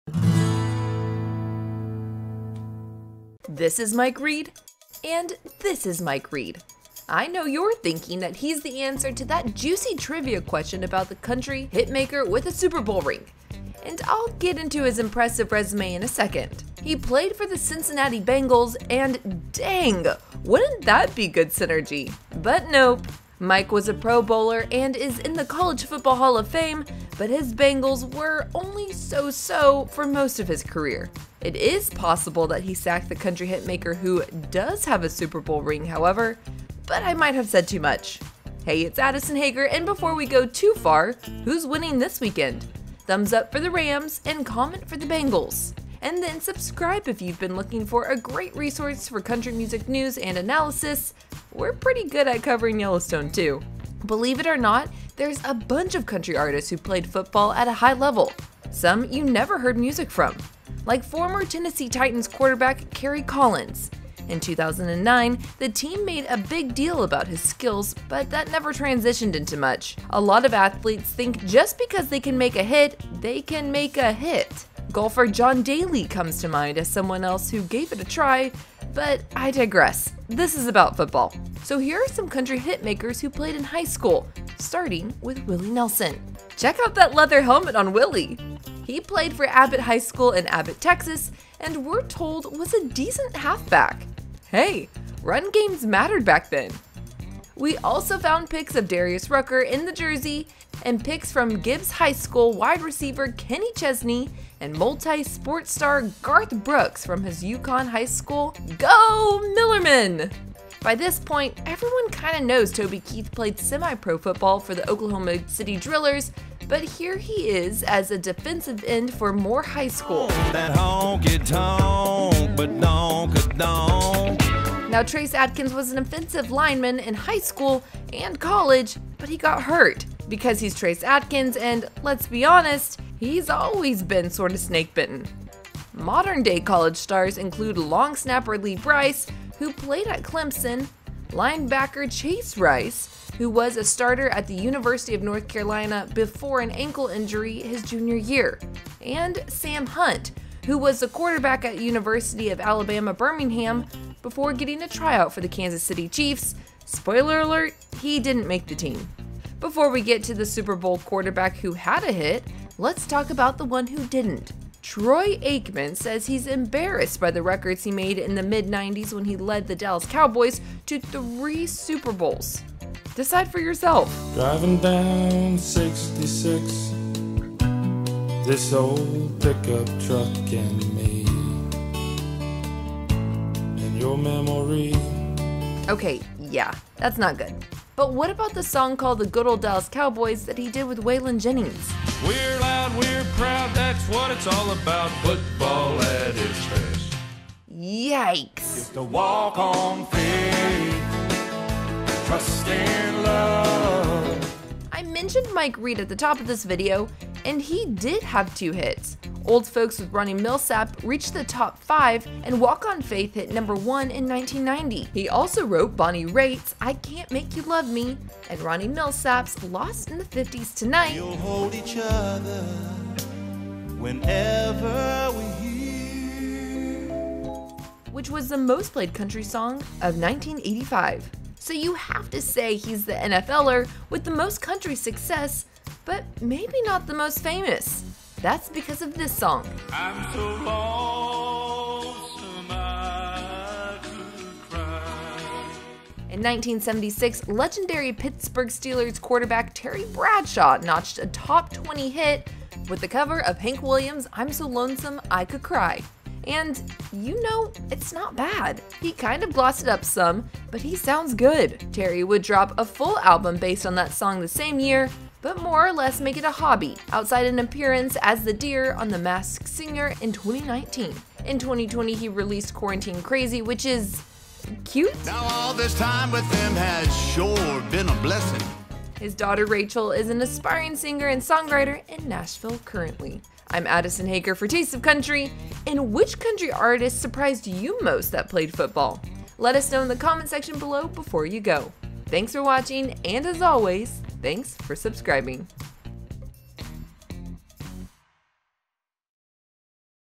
This Country Hitmaker Is Also a Super Bowl Champion
An NFL legend and Super Bowl champion scored a big hit on country radio with his cover of a Hank Williams song. Do you remember?

Country music and football go together like mashed potatoes and gravy so it's not surprising to hear so many singers have played football. Several football players have given country music a go too, with at least two of them notching big hits.

The first had two huge songs in the late 1980s after his Pro Bowl career wrapped with the Cincinnati Bengals. The second notched a single Top 20 in the mid-70s, but he remains very famous today.

Just for fun we added football pictures from Willie Nelson, Toby Keith, Kenny Chesney and more and included Troy Aikman's attempt at a country song. Yeesh, it's ummm, well decide for yourself.

0:00 | Country singers who played in the NFL
1:26 | Willie Nelson played ball
1:42 | Kenny Chesney had good hands
2:05 | Trace Adkins was great, but then …
2:31 | The country QB who tried out for the Chiefs
2:45 | It’s true, Troy Aikman tried country music!
3:21 | The NFL’s only country chart-topper
4:03 | Country music’s Super Bowl champ